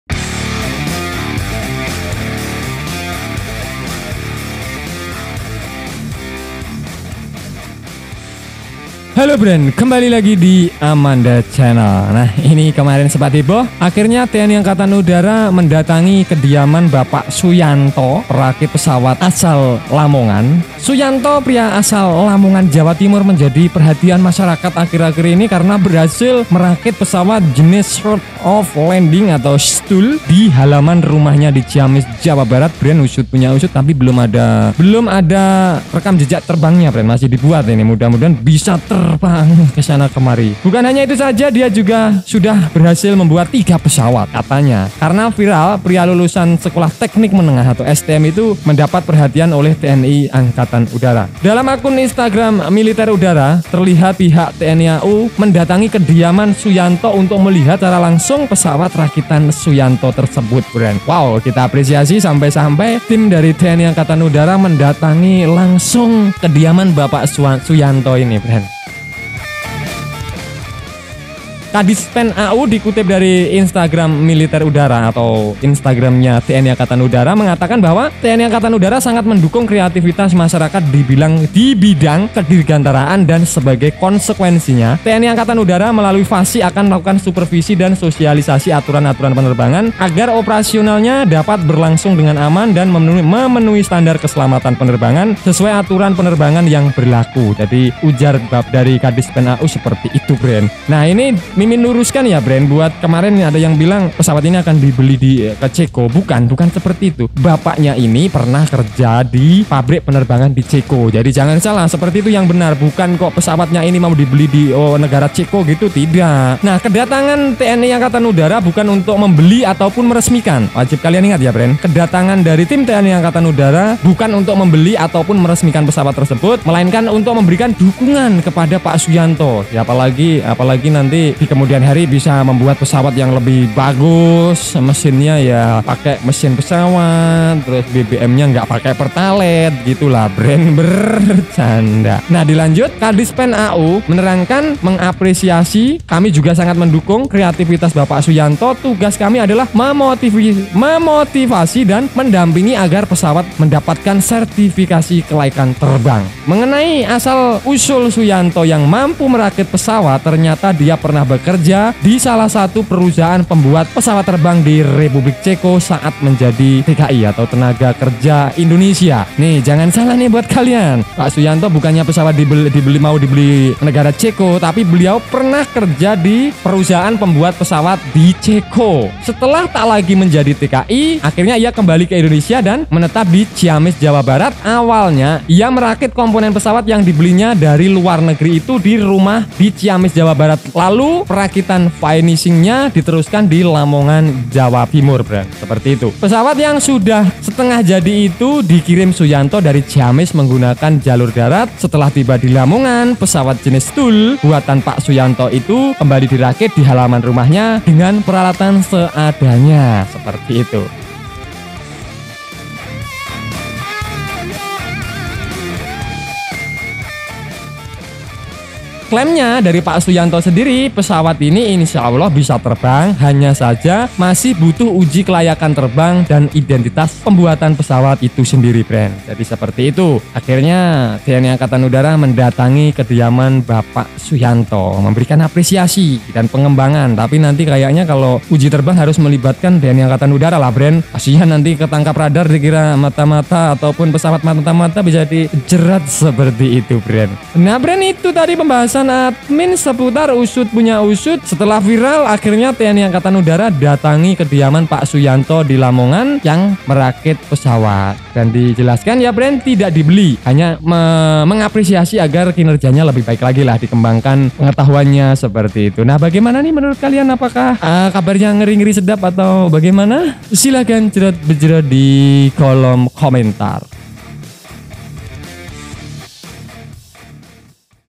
Ada Apa Nih, TNI AU Sidak Rumah Suyanto Pembuat Pesawat Lamongan
Suyanto, pria asal Lamongan, Jawa Timur, menjadi perhatian masyarakat karena berhasil merakit pesawat jenis short off landing (STOL) di halaman rumahnya di Ciamis, Jawa Barat.

Bukan hanya itu saja, dia juga sudah berhasil membuat tiga pesawat. Karena viral, pria lulusan Sekolah Teknik Menengah (STM) itu mendapat perhatian dari TNI Angkatan Udara.

Dalam akun instagram @militerudara, terlihat pihak TNI AU mendatangi kediaman Suyanto untuk melihat secara langsung pesawat rakitan Suyanto.

" TNI AU sangat mendukung kreativitas masyarakat dibidang kedirgantaraan. Dan sebagai konsekwensinya, TNI AU melalui FASI, akan melakukan supervisi dan sosialisasi aturan-aturan penerbangan, agar operasionalnya dapat berlangsung dengan aman dan memenuhi standar keselamatan penerbangan sesuai aturan penerbangan yg berlaku," ujar Kadispenau dikutip dari Instagram @militer.udara.